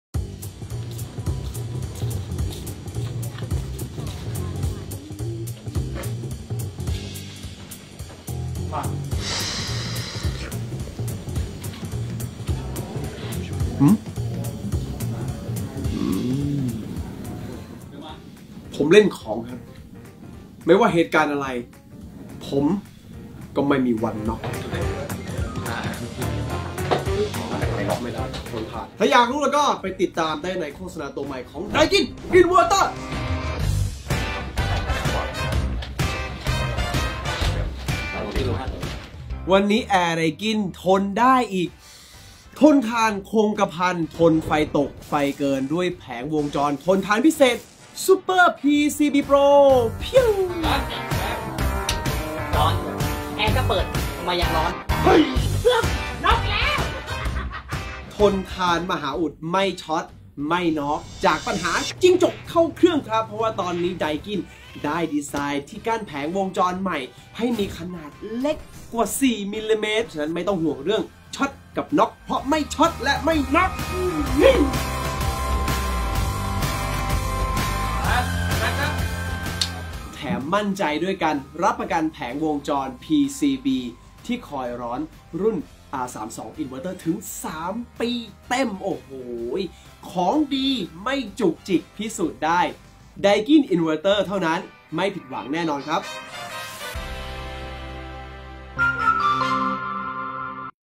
ณเดชน์กลับมาอีกครั้ง! คราวนี้พกของดีมาด้วย ทั้งทนทาน คงกระพัน ด้วยแผงวงจรใหม่สุดพิเศษ
ณเดชน์กลับมาอีกครั้ง!
คราวนี้พกของดีมาด้วย ทั้งทนทาน คงกระพัน
ด้วยแผงวงจรใหม่สุดพิเศษ

ดูเวอร์ชั่นเต็มได้ที่นี่เลย